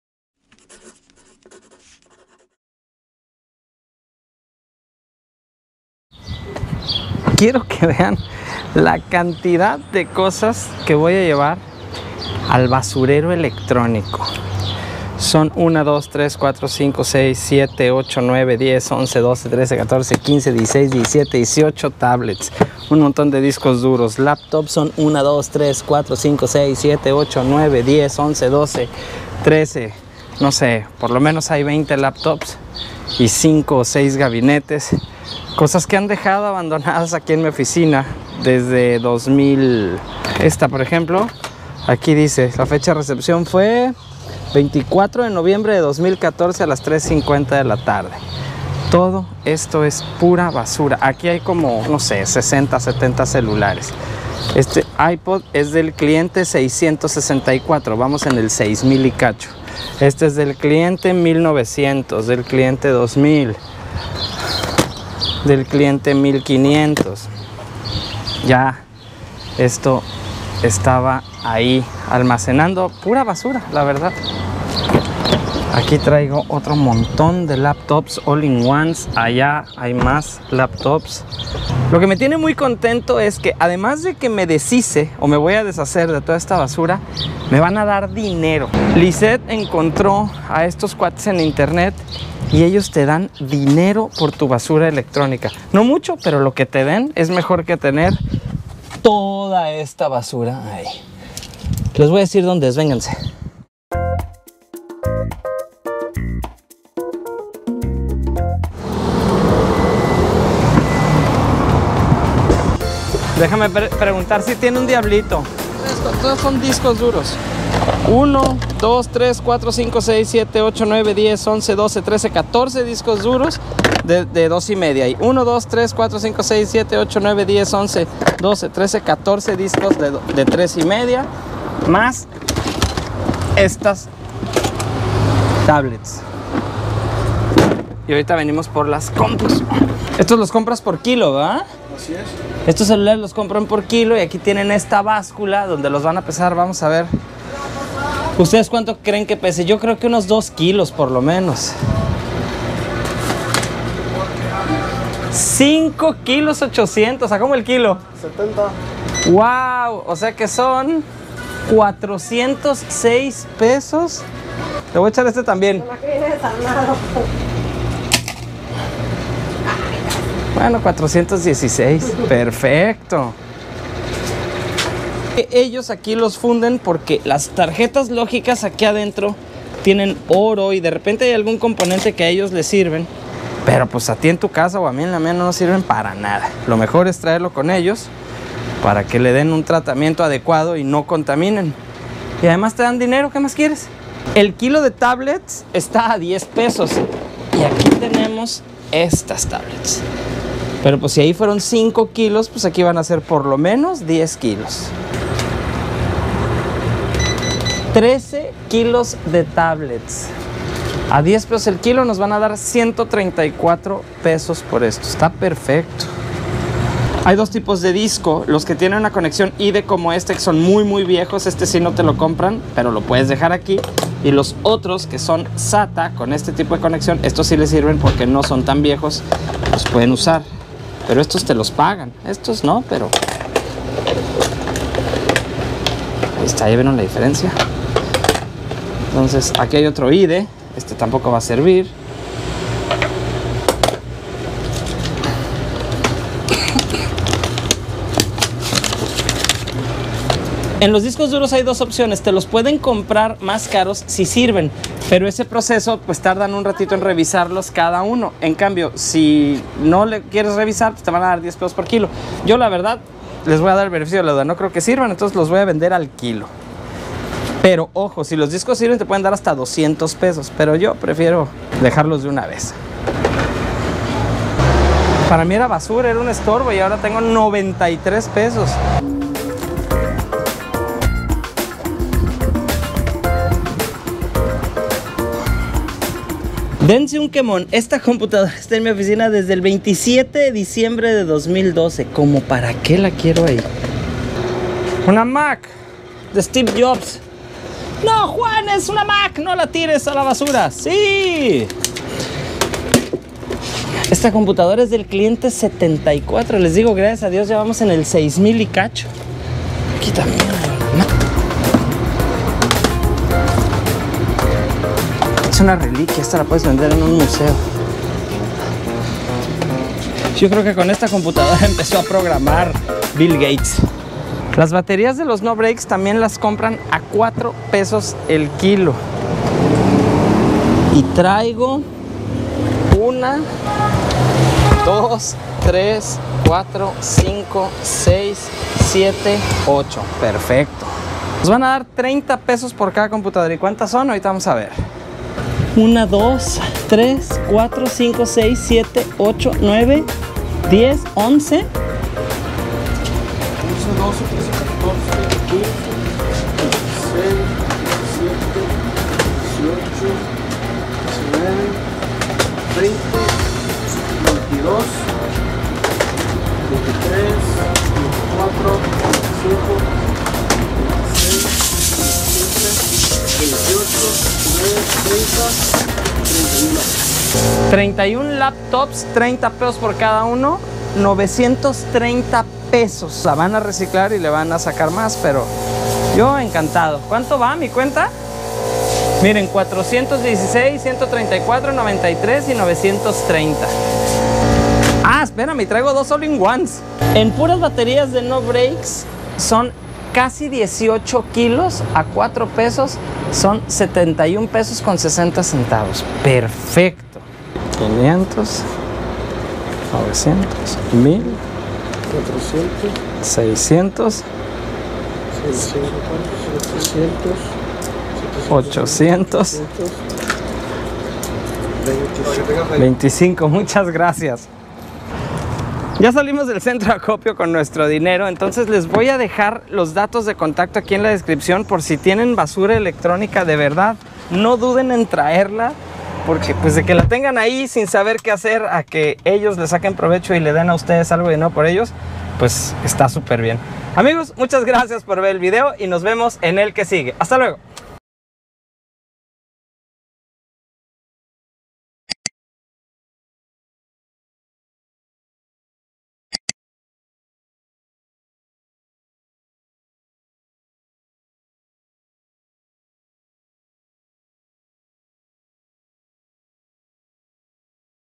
Gané dinero por llevar “basura” | JuanDeAvilaMx
¿Tienes una computadora vieja, un celular que no sirve una Tablet que sale más caro arreglarla que comprar otra?  Pues te invito a que busques en casa de tus vecinos, amigos y lugares aledaños, porque entre ayer y antier yo me “gane” más de $2,500 pesos de solo por llevar decenas de computadoras, celulares y discos duros que no servían.  Busca en tu ciudad quien compra electrónicos descompuestos.